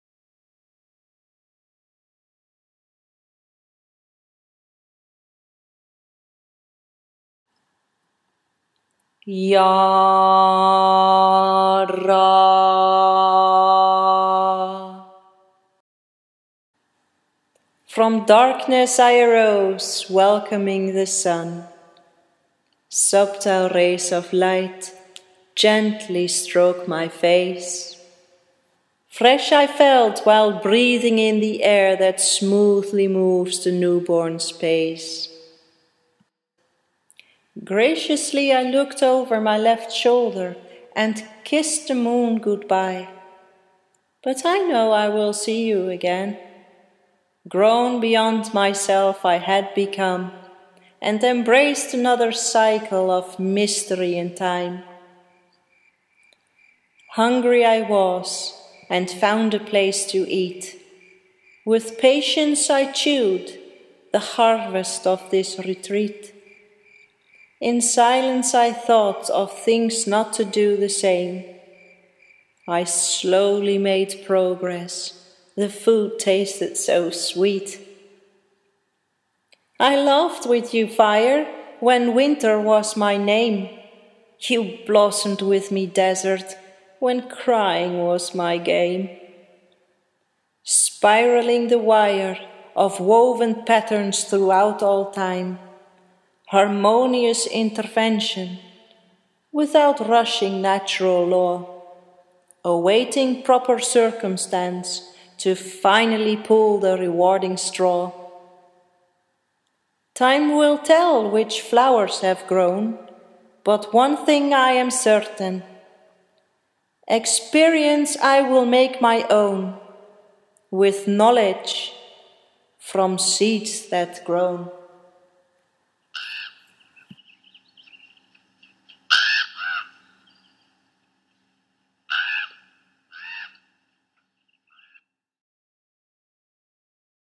Rune Power: The cycle of change; Jara, Jera ( Elder Futhark )
Rune Poem Written, Video Editing and Narrator by: Sigha Manning

Jara is the rune of harvesting the outcome of past efforts made. Rewards for positive action / mindset through out time.

farming, waiting ,seasons, harvest, activity of the mind, predicting outcome, patience, year, spiraling the flow of life.

The way out is to go Within